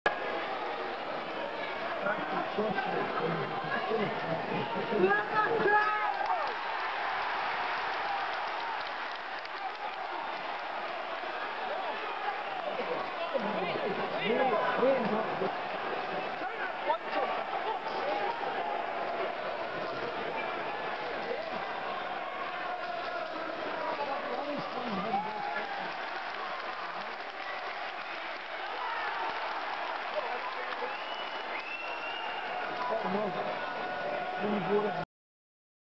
[Fan Video] 'The Rickster' Ricky Burns v Roman 'Rocky' Martinez clip
A short clip of the unbelievable night at The Kelvin Hall, Glasgow when Ricky Burns dethroned the dangerous champion Roman Martinez of Puerto Rico to capture the WBO Super Featherweight title.